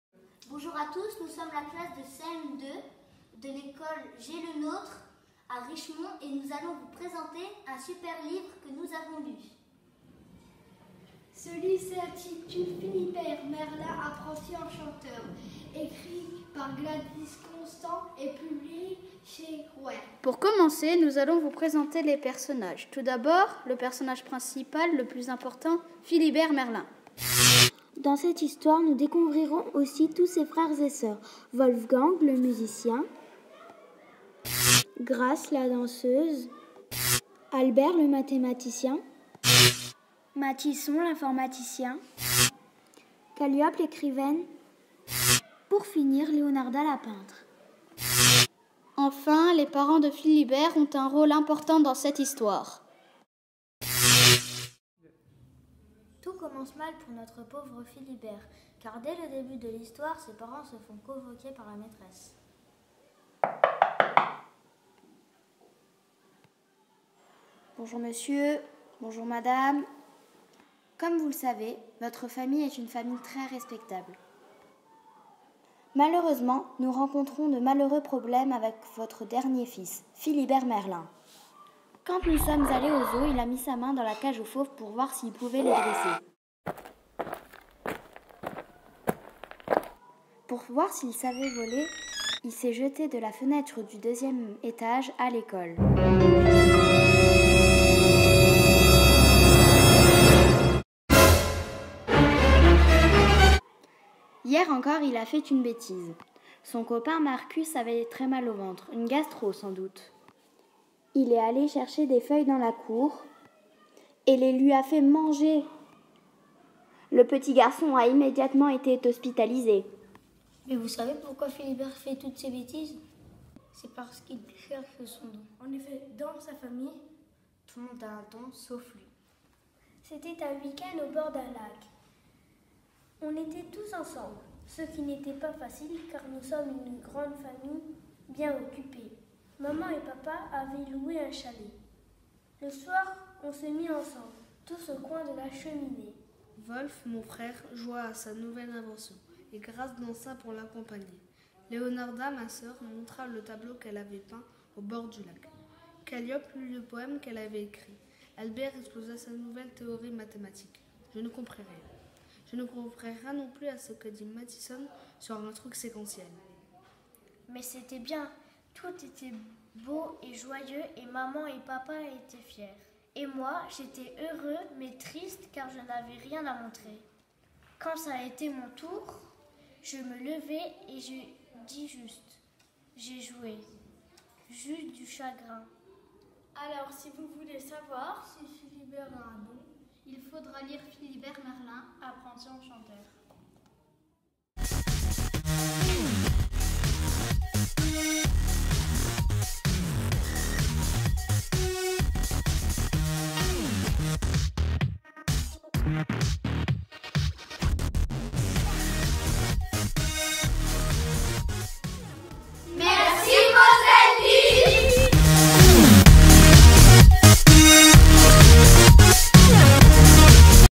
Mosellire 2019 Ecole Lenotre de Richemont Cat CM1 CM2 2e prix partenariat
Dans le cadre du concours départemental des jeunes lecteurs MoseL’lire 2018/2019, les participants pouvaient présenter un livre de la sélection sous la forme d’une vidéo, à la manière de booktubeurs.

Parmi les productions vidéos lauréates de cette 12e édition, voici le booktube de l'Ecole Lenotre de Richemont Cat  CM1 CM2   2e prix partenariat.